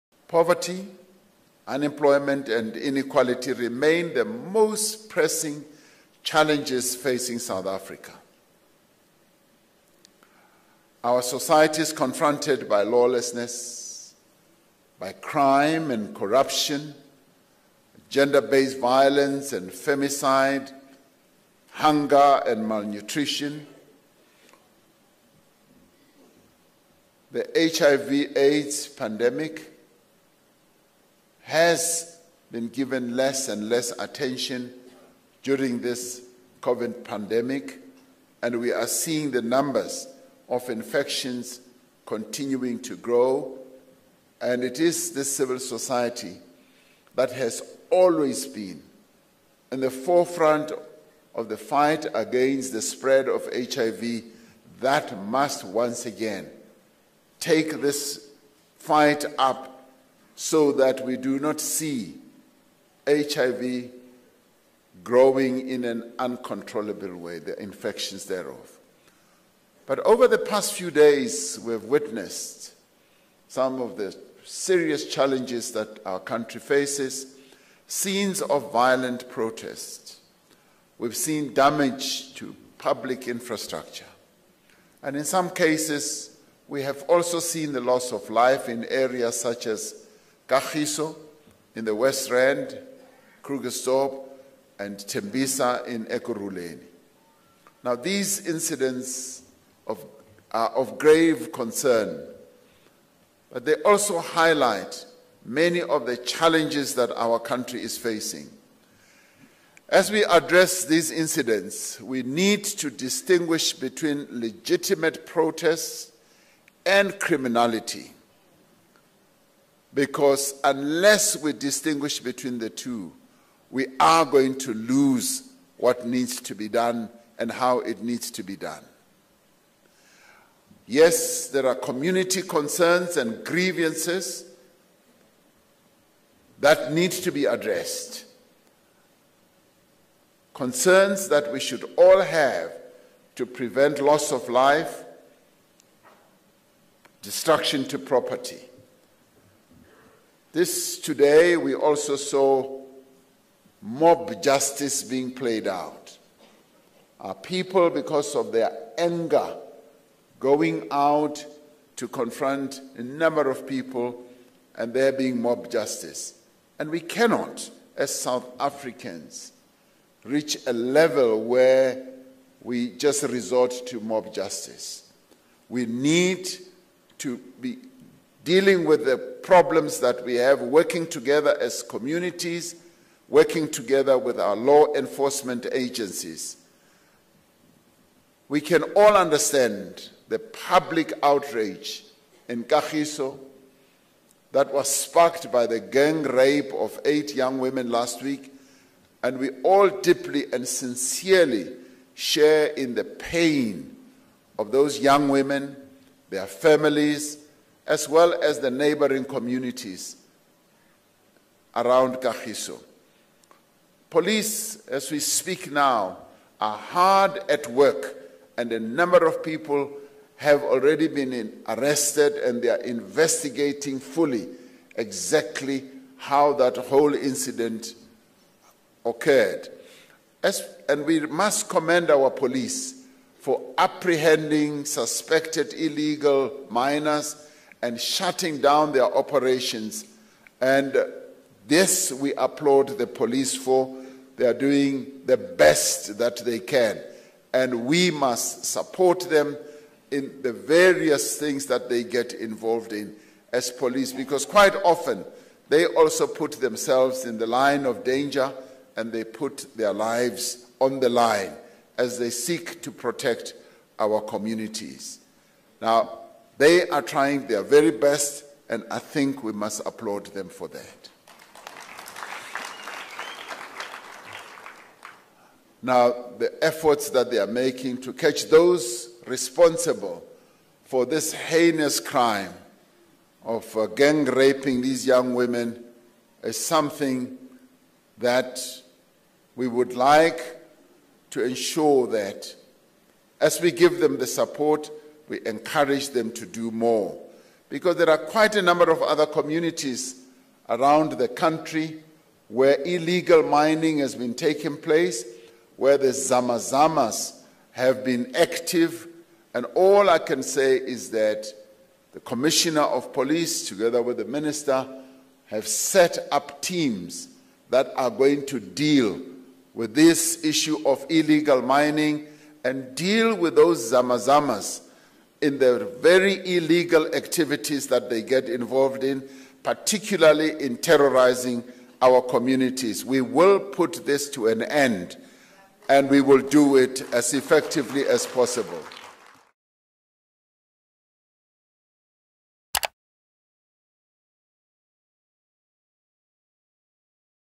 Saps, Mineral Resources dept to address illegal mining, crime in Krugersdorp – Ramaphosa
President Cyril Ramaphosa announced on Friday that government, represented by the South African Police Service (Saps) and the Department of Mineral Resources and Energy, will convene an Imbizo with the Kagiso community over the weekend to interrogate issues of illegal mining and crime in Krugersdorp.